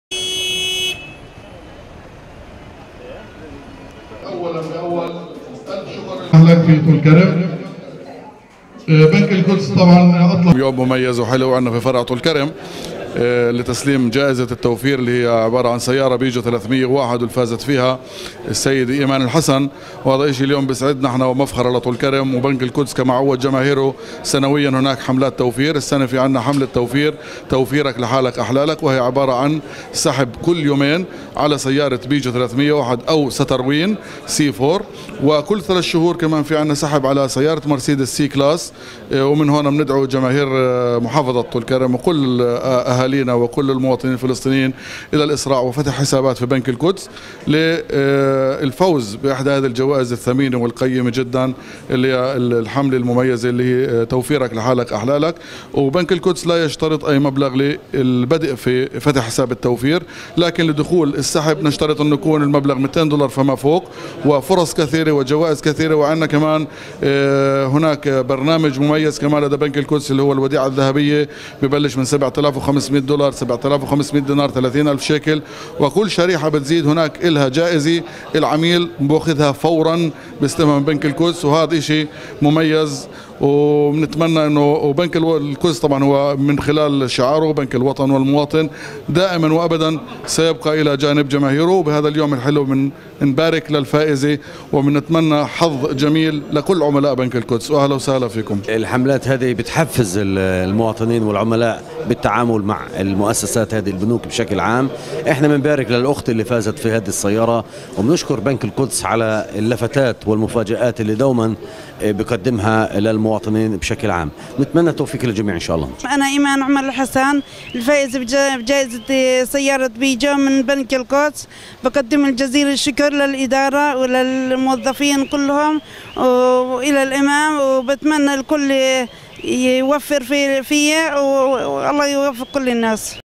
بنك القدس يسلم سيارة بيجو للفائزة في حملة توفيرك لحالك احلالك
تلفزيون الفجر الجديد 11/5/2016 | بنك القدس يسلم سيارة بيجو للفائزة في حملة توفيرك لحالك احلالك